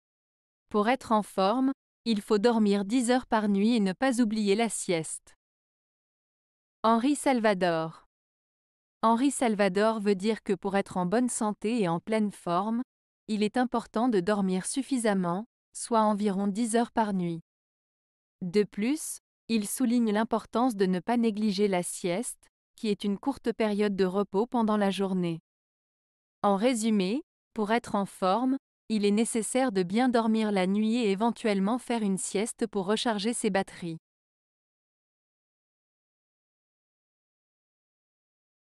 Henri Salvador - Pour être en forme, il faut dormir dix heures par nuit et ne pas oublier la sieste.
Henri Salvador veut dire que pour être en bonne santé et en pleine forme, il est important de dormir suffisamment, soit environ dix heures par nuit. De plus, il souligne l'importance de ne pas négliger la sieste, qui est une courte période de repos pendant la journée. En résumé, pour être en forme, il est nécessaire de bien dormir la nuit et éventuellement faire une sieste pour recharger ses batteries.